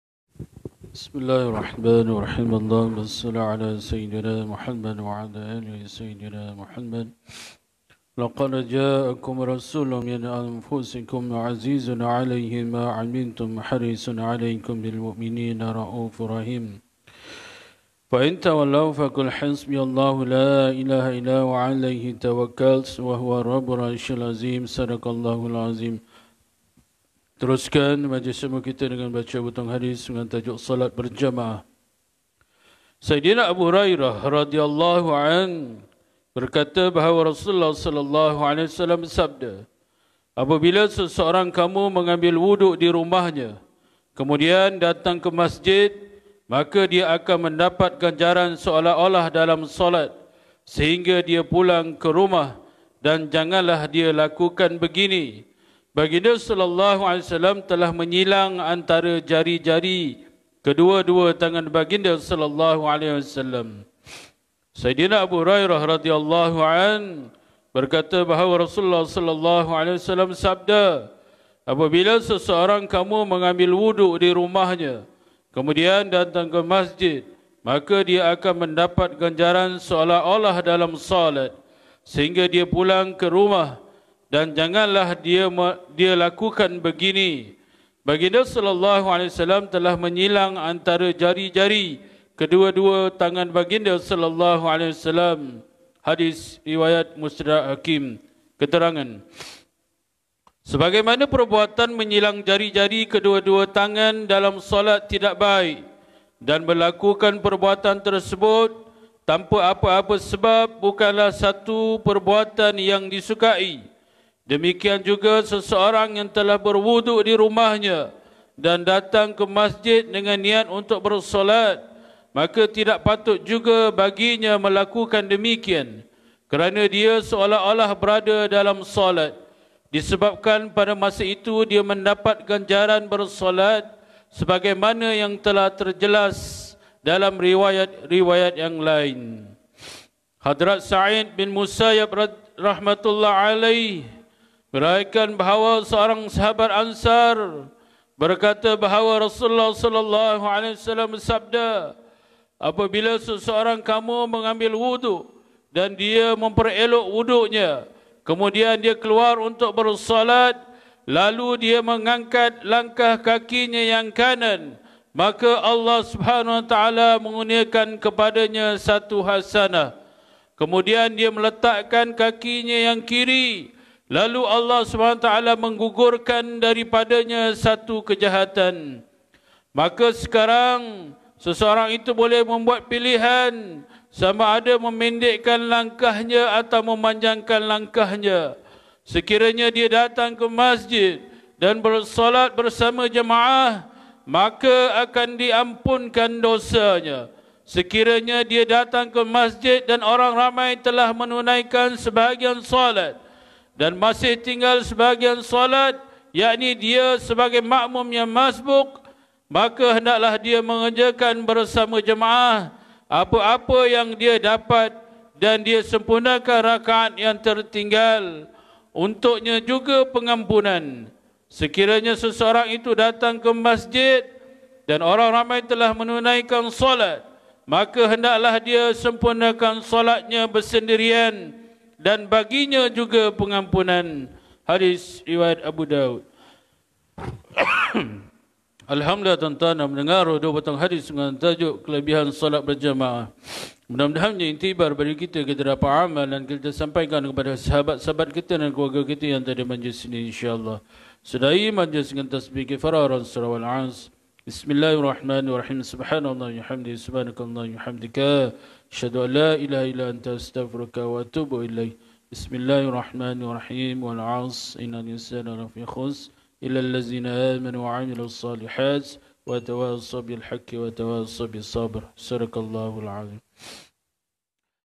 MAJLIS TA'LIM (30-01-2021) - SOLAT BERJEMAAH
بِسْمِ  ٱللَّهِ ٱلرَّحْمَـنِ ٱلرَّحِيم

السَّلاَمُ عَلَيْكُمْ وَرَحْمَةُ اللّهِ وَبَرَكَاتُهُ

MAJLIS TA'LIM

Bacaan Hadith dengan tajuk SOLAT BERJEMAAH pada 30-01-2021 selepas Solat Subuh bertempat di Surau Al-Mizan Kg. Sagil Pt. 3, Tangkak Johor.

Semoga beroleh manfaat kepada yang menonton dan yang menyebarkannya.

Sumbangan Ikhlas ke Akaun Surau Al-Mizan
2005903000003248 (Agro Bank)
Terima kasih.